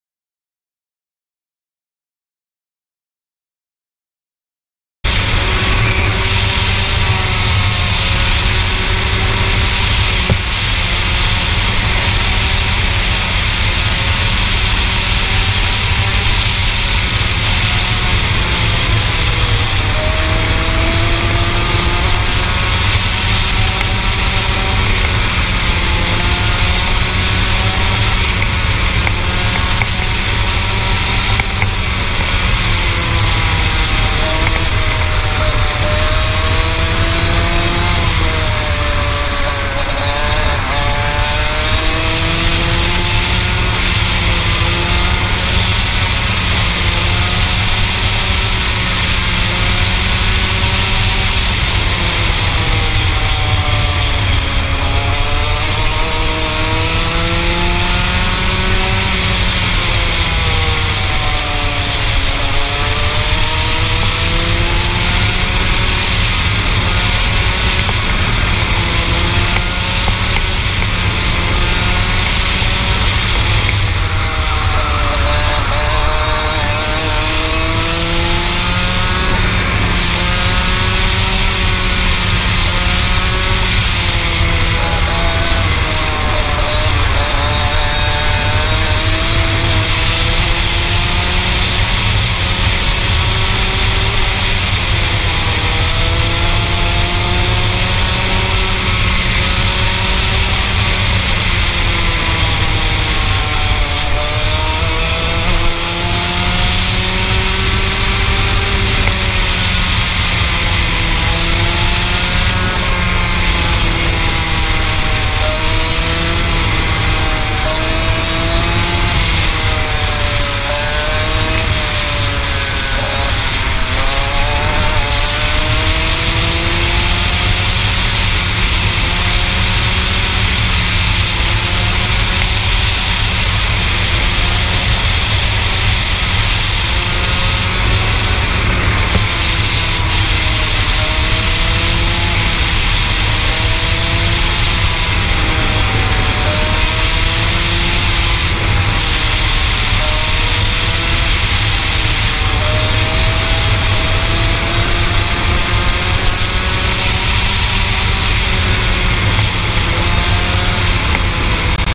KARTING : ROTAX MAX 125CC FULL THROTTLE l AIGINIO CIRCUIT
Driver : John Moumos
Engine : Rotax Max 125FR
Chassis : TOP KART 2010
Camera : Go Pro Hero 4
Driving in abandoned circuit in Greece.